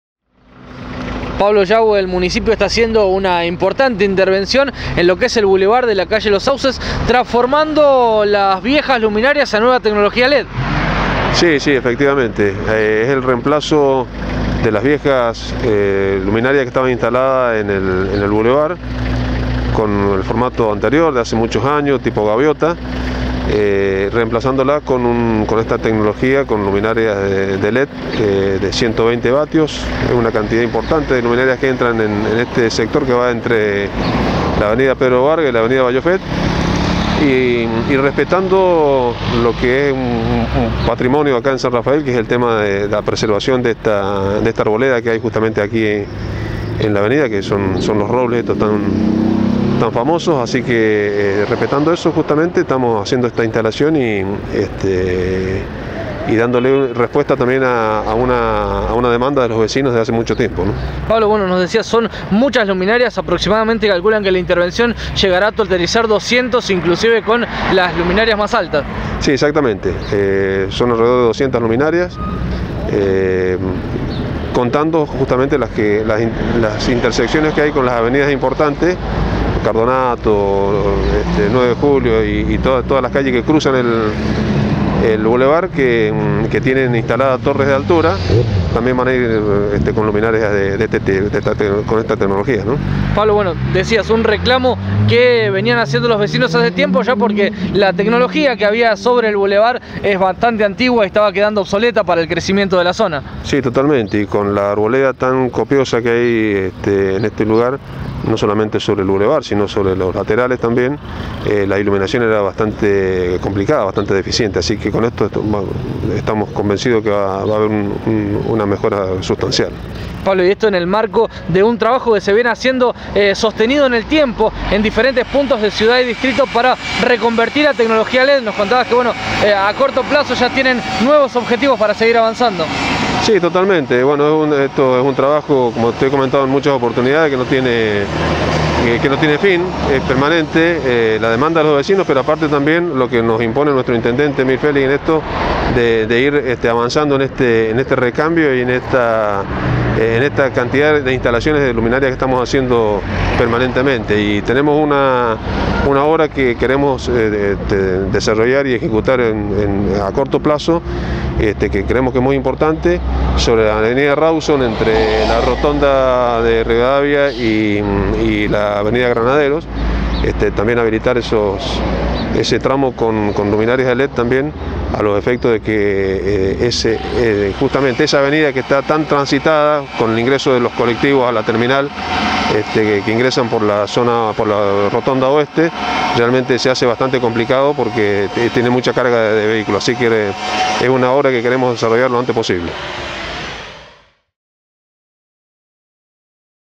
NUEVAS LUCES LED EN EL BULEVAR DE AVENIDA LOS SAUCES🟢SanRafael
Pablo Yagüe, Director de Electrotecnia

El área de Electrotecnia trabaja con la colocación de 200 nuevos equipos a lo largo del bulevar “Los Robles” que se extiende sobre Avenida Los Sauces desde Pedro Vargas a Balloffet.